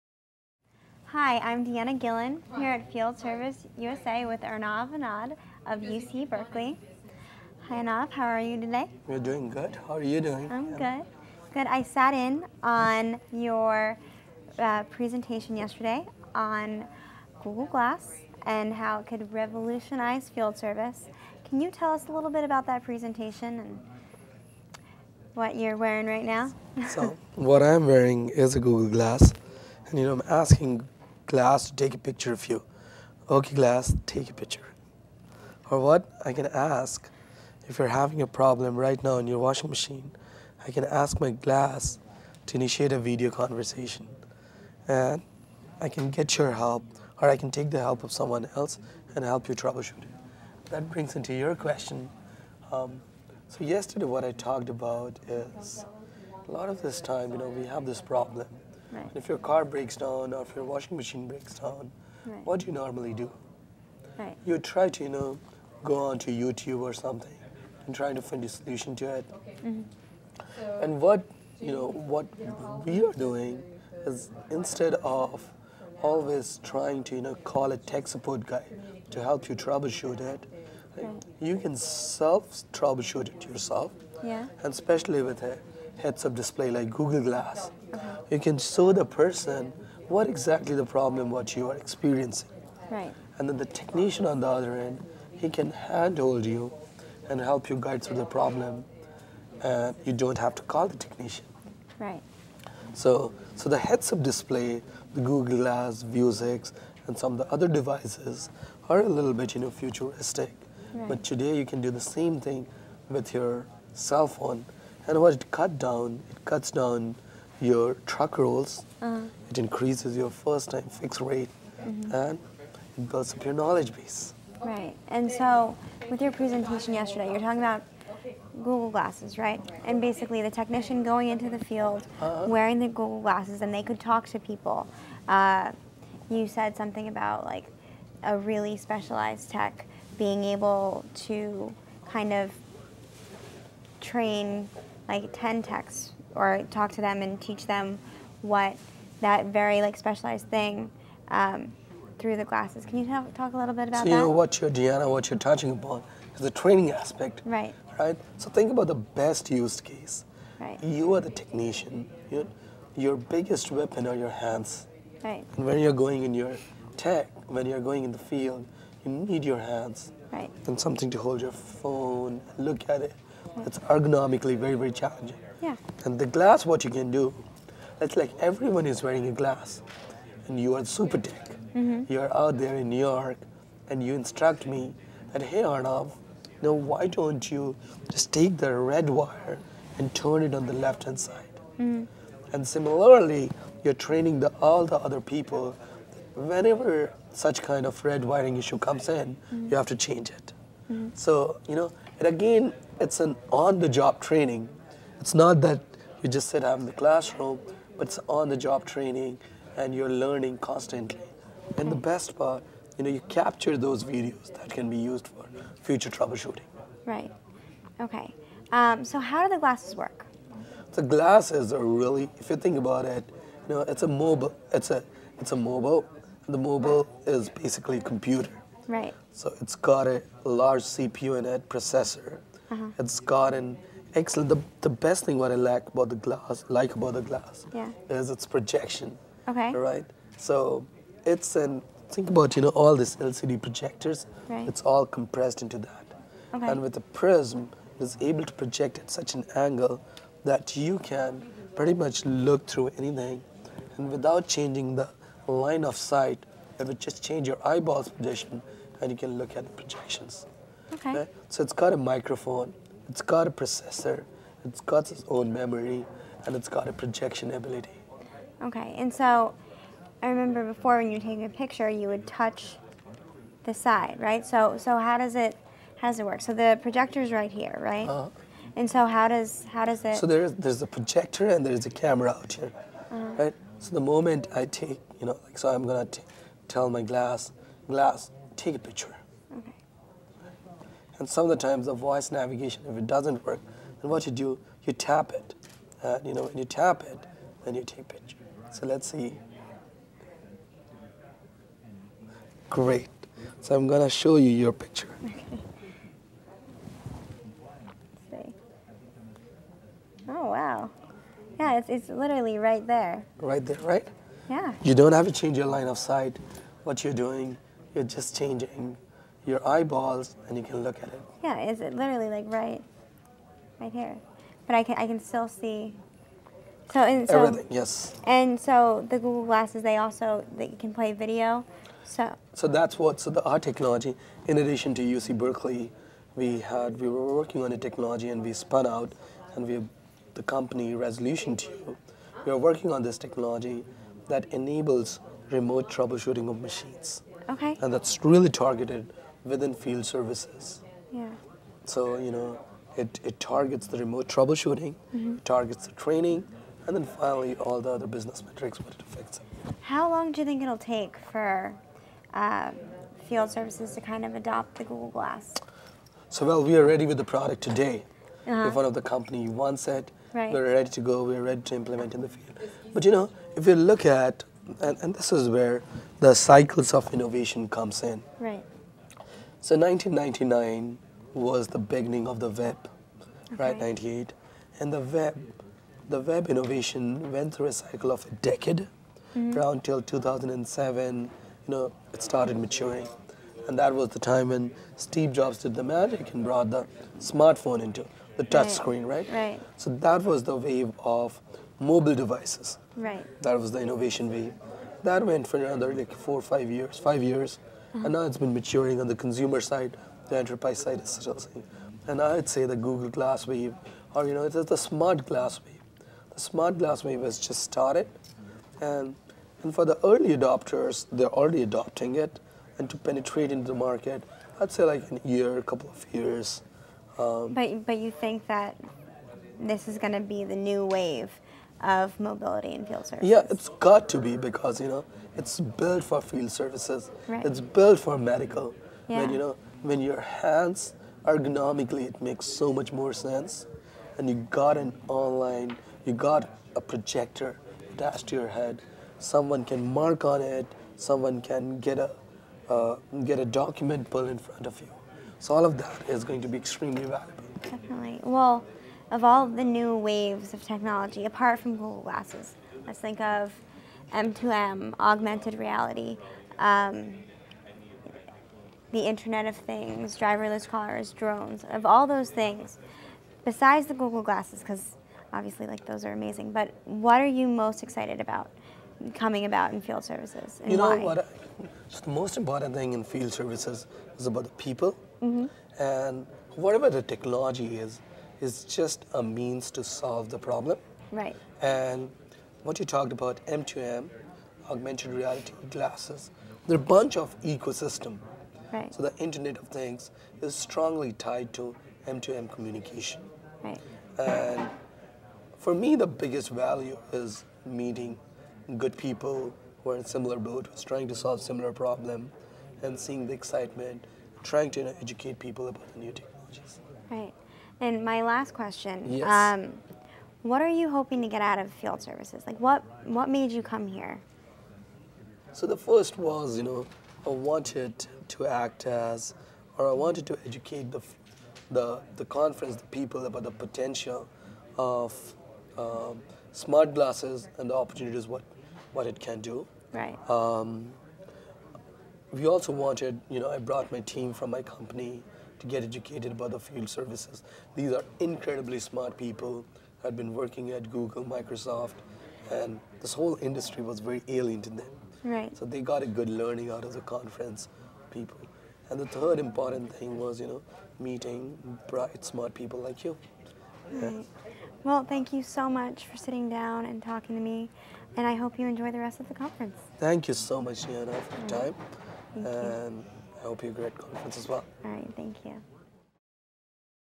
Arnav Anand of UC Berkeley on the Future of Field Services and Google Glass
Arnav Anand, Instructor & Entrepreneur-in-Residence (EIR) at Center for Entrepreneurship and Technology, UC Berkeley discusses to future of field services and Google Glass.